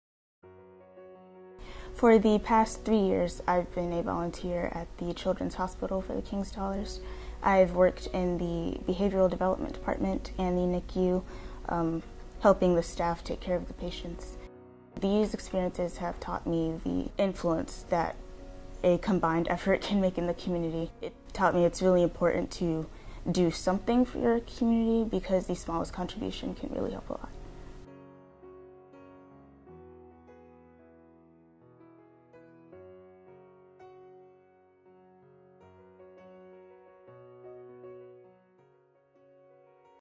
Experience is the best teacher: Community Service Lessons
Hear from King's Fork High School's 2018 Salutatorian LaDariah Boone about her experiences volunteering at Children's Hospital of the King's Daughters.